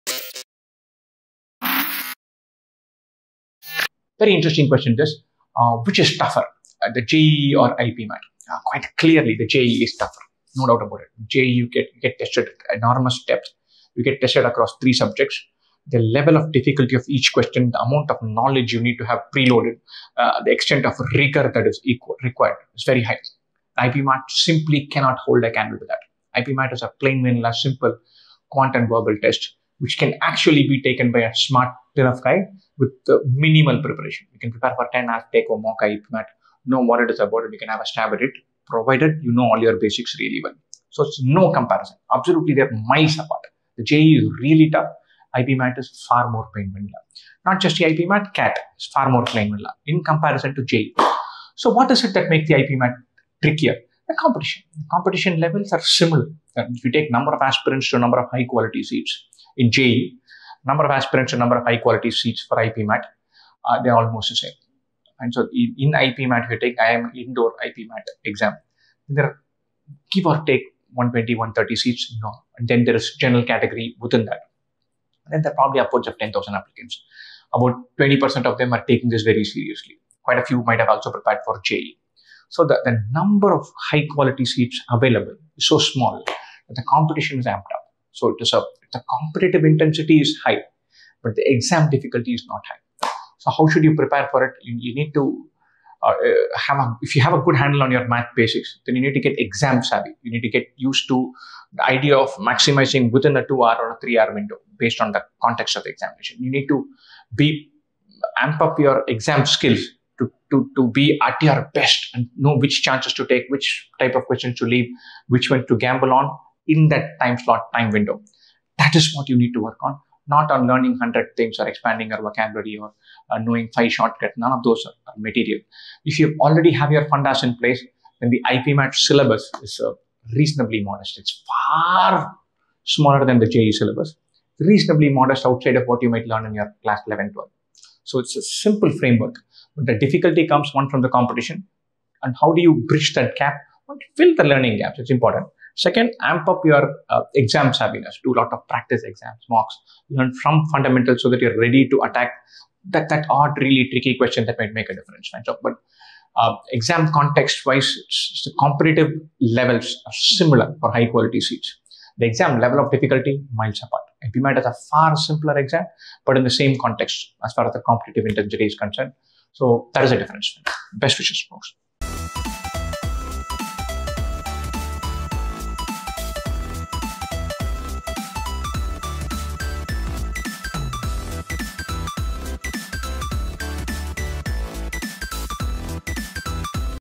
JEE vs IPMAT - Which is tougher? | IPMAT and JIPMAT |2IIM IPMAT Preparation
Hi Folks,

If you're planning to take up IPMAT and wanted to know how it compares to JEE, Then this is the video for you.

How tough is IPMAT when compared to JEE?
What should you do differently for this exam?
How to prepare for it?

All similar questions answered in this video. Watch the video Now!

Best wishes for IPMAT!